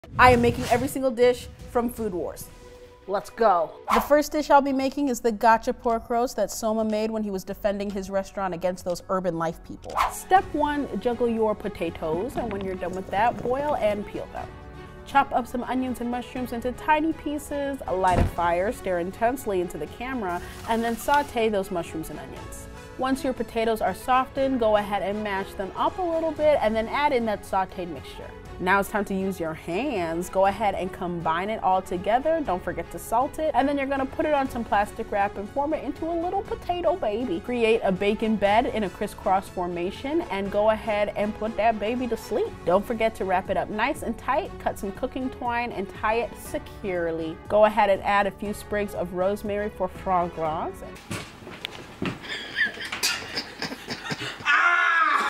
Gotcha Pork Roast from Food Wars
I Made Every Single Dish From Food Wars Part 1

Élise is back with the Anime foods! Today's challenge...Food Wars!